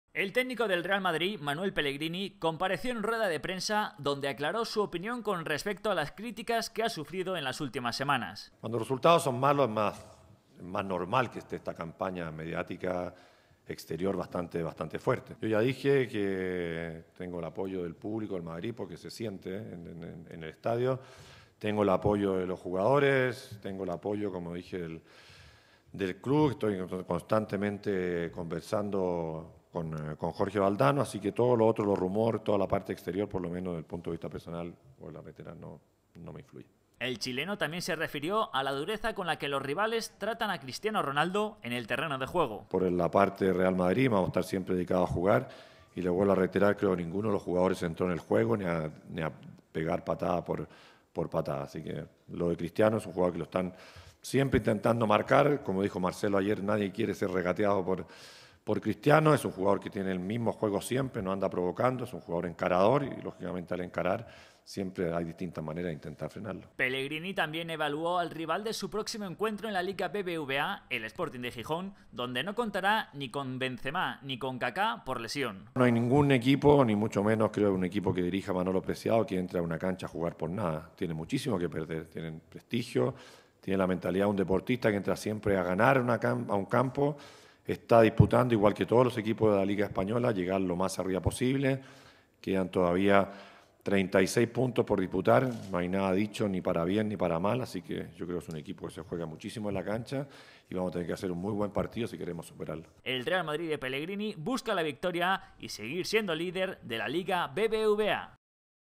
Pellegrini: "No me afecta la campaña mediática"
El técnico del Real Madrid evaluó las críticas que recibió en las últimas semanas, así como la dureza con que se trata a Cristiano Ronaldo en el terreno de juego. El chileno también se refirió a su próximo rival en la Liga BBVA, el Sporting, ante el que no contará ni con Benzema ni con Kaka'.